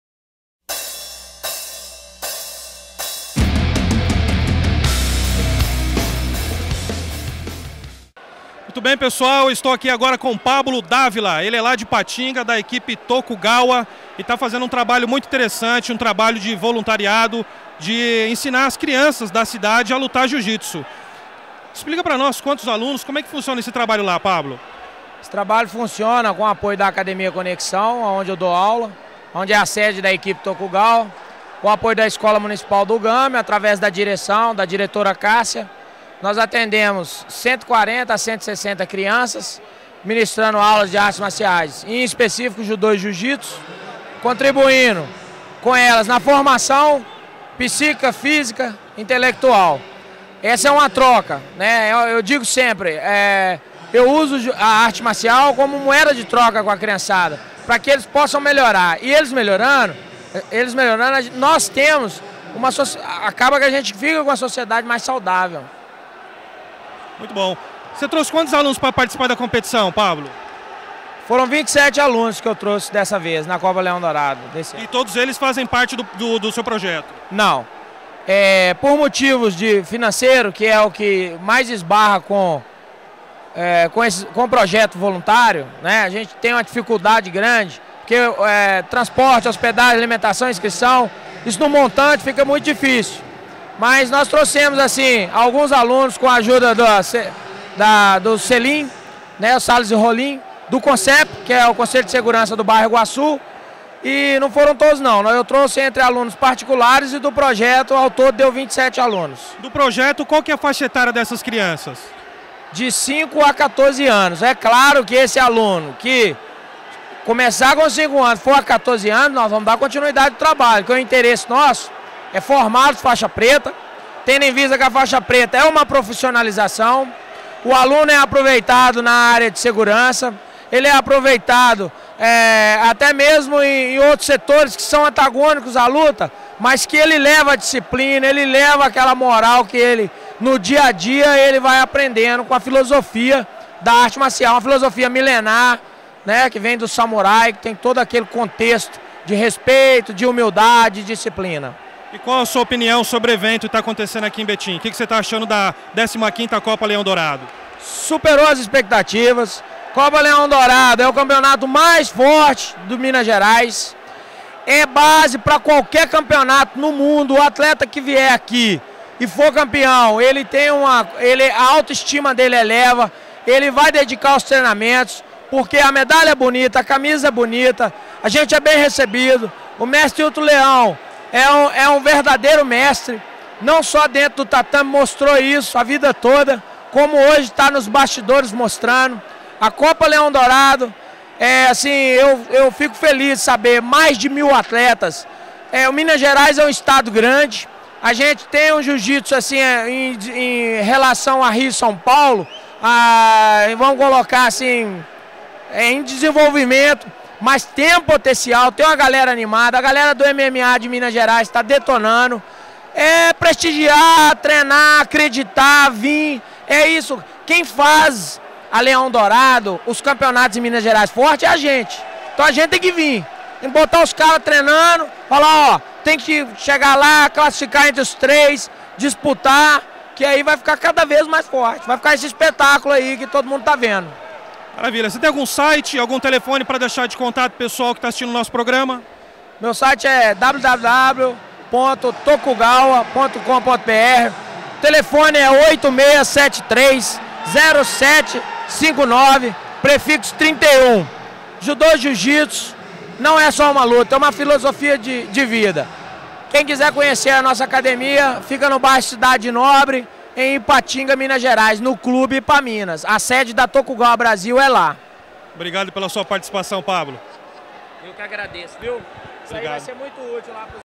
Responsável pela equipe Tokugawa fala sobre projeto que ajuda crianças carentes
A equipe Tokugawa Jiu-Jitsu e Judô foi fundada pelo professor Pablo D'avila em 1996, na cidade de Ipatinga Minas Gerais. A Tokugawa foi a pioneira do Jiu-Jitsu no Vale do Aço, e hoje conta com inúmeros títulos regionais, estaduais, nacionais e internacionais na modalidade Jiu-Jitsu. Hoje a sede da equipe Tokugawa Jiu-Jitsu e Judô atualmente é na academia Conexão no clube Ipaminas, em Ipatinga.